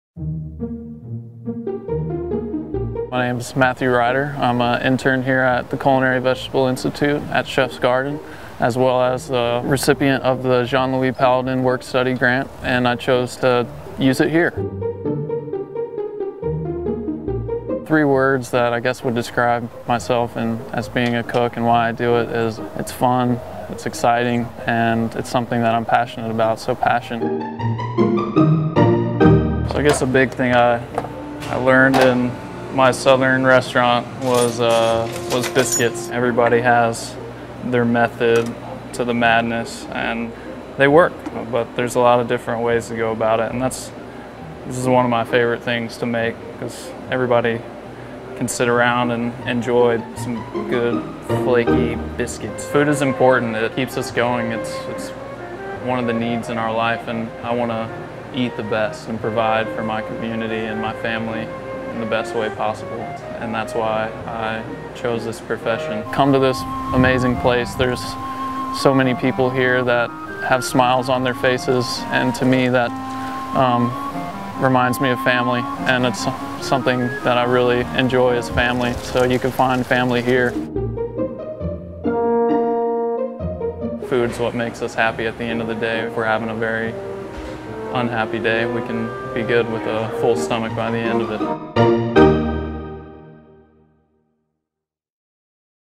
Chef Matt Ryder's Biscuits
Join us for an evening with culinary intern Matt Ryder. Experience plates inspired by Native American, Hispanic, Cuban and Southern American influences melded with a new culinary sensibility influenced by his internship at the CVI with Chef Jamie Simpson. Each plate will be evocative of Matt's life experiences in New Mexico, Florida, North Carolina and now Ohio – each reinterpreted to reflect Matt's emerging, distinctive style.

Matt Ryder is this year's recipient of the coveted Jean-Louis Palladin Work Study Grant awarded by the James Beard Foundation to young professional chefs to learn alongside master artisans, producers, and fellow chefs in pursuit of the same high standards for ingredients that characterized Palladin's philosophy and personal cuisine.

Doors open at 6:30 pm for Cocktails, 7:00 pm Dinner

Limited Availability.  Reserve your seat today!

Photography is permitted, and sharing is encouraged, but audio or video recording is not allowed.